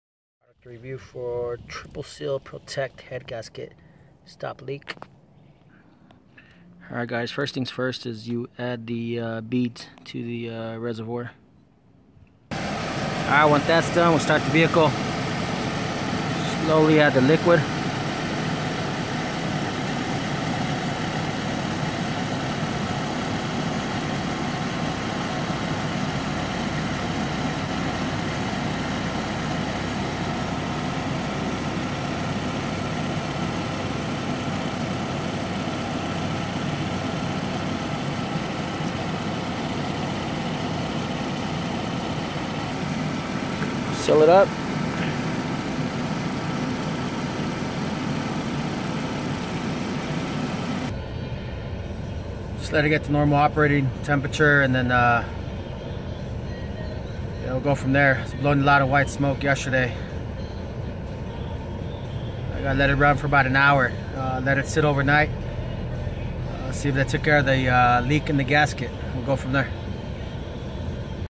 How to seal head gasket. Saturn ion Diy
Prestone head gasket stop leak. 2005 saturn ion 2.2l engine.